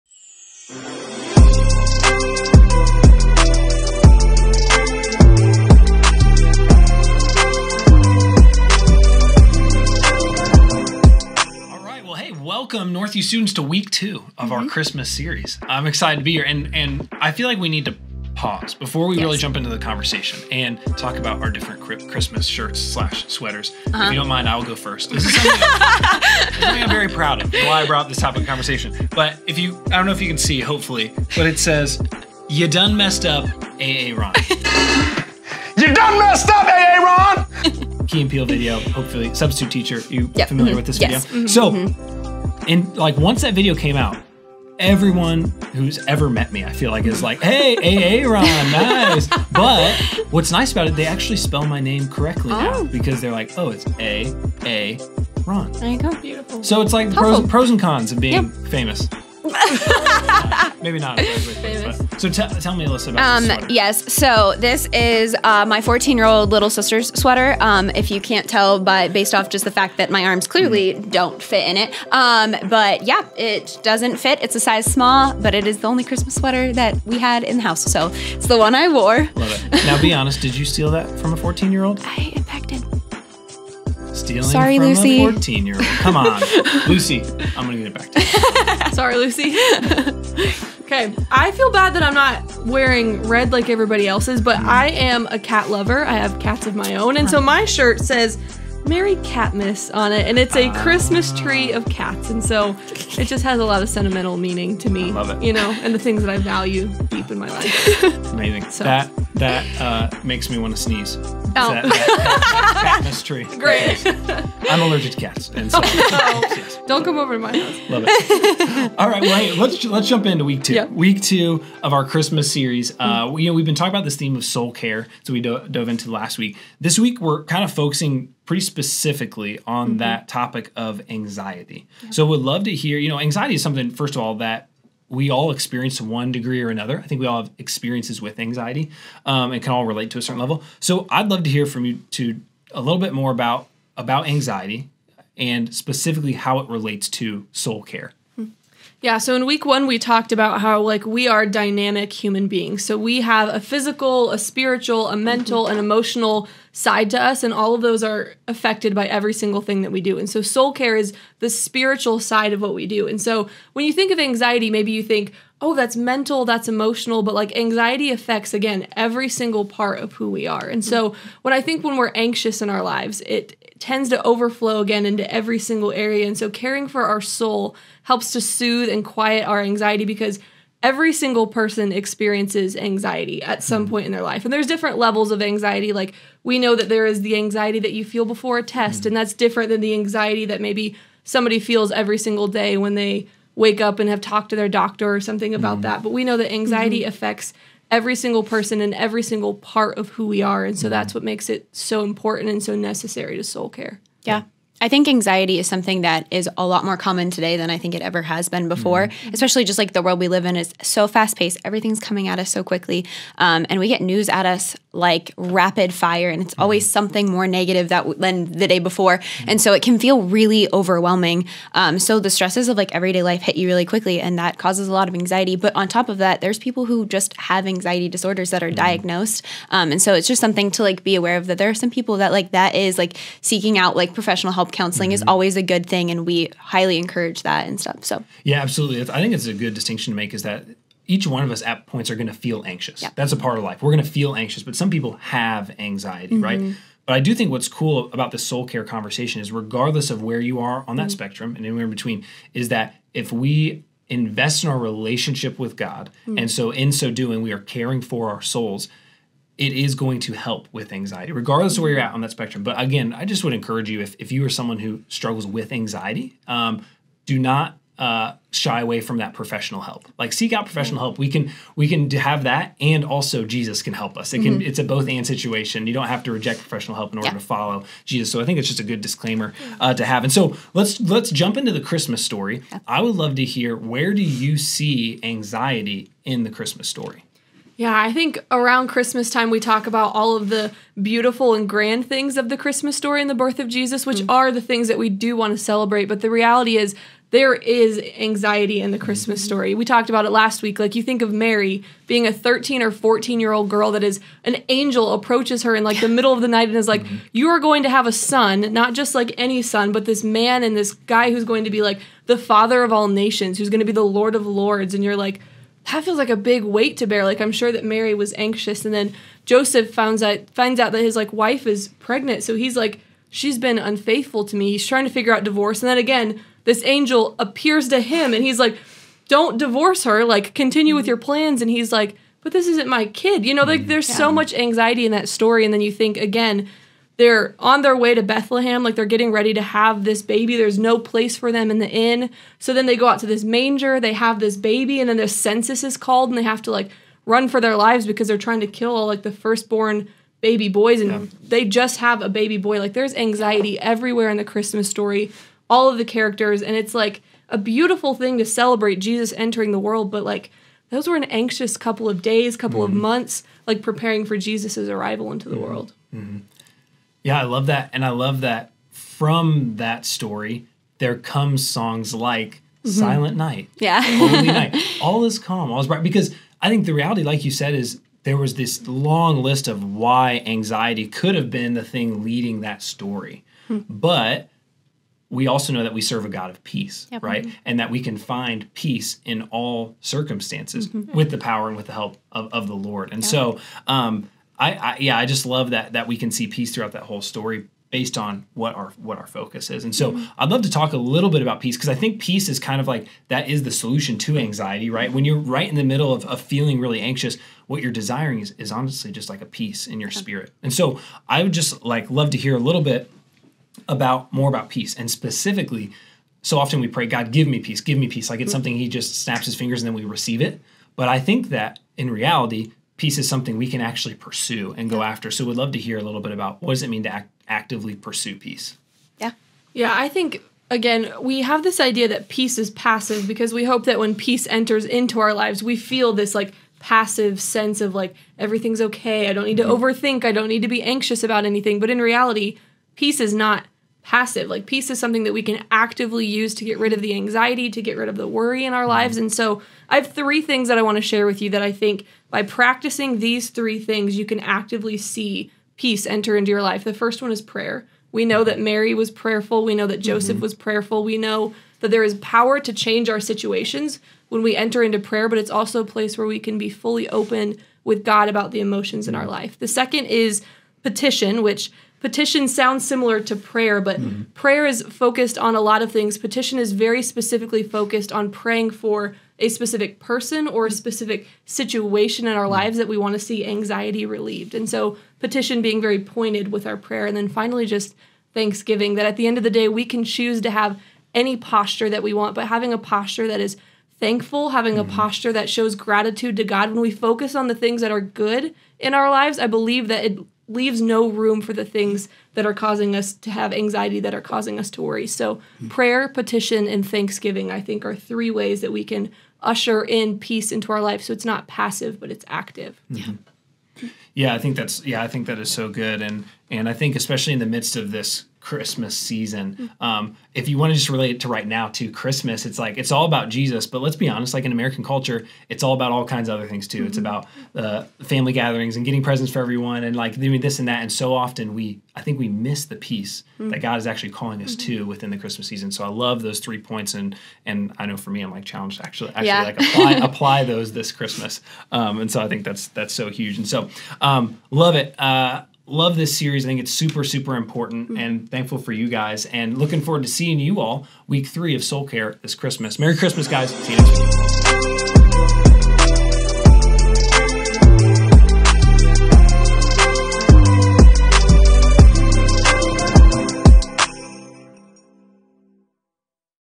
NVS LIFE GROUPS | SOUL CARE | WK 2
Series Overview: This series will focus on soul care using the story of the birth of Jesus. We will be breaking down the verse Romans 15:13 to better understand why it is important to take care of our soul. Going through the topics of seeing emotions as a gift and knowing how to process them, dealing with anxiety, and seeing the need for hope in Jesus in what feels like a hopeless world.

What does it mean to be “filled with all peace” when anxiety is constantly surrounding us from all sides? Through the lens of the Christmas story, we are going to look at how God has given us the weapon of peace.

Scripture: Romans 15:13, Luke 1:26-33, Matthew 1:19 Luke 2:6-7, Matthew 2:13-15, Philippians 4:4-7

THP (Take Home Point): The Peace of God Transcends All Understanding